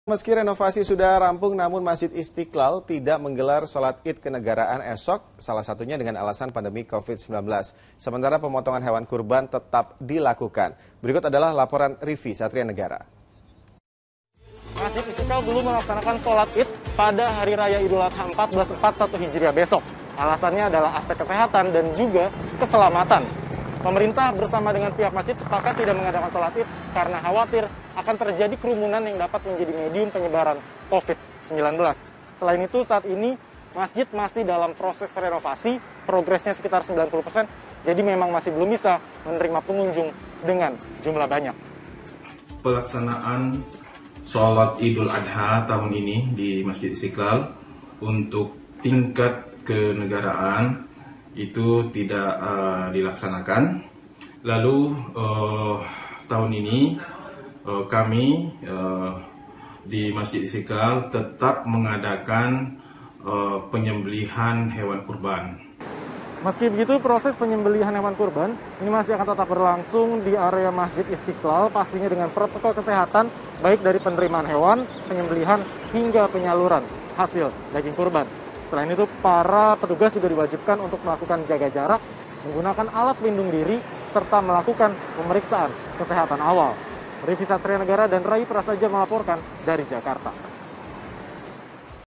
Meski Sudah Bersolek, Masjid Istiqlal Jakarta Masih Belum Menggelar Salat Iduladha
Meski renovasi sudah rampung, namun Masjid Istiqlal tidak menggelar Salat Id kenegaraan, salah satunya dengan alasan pandemi covid-19. Sementara pemotongan hewan kurban tetap dilakukan.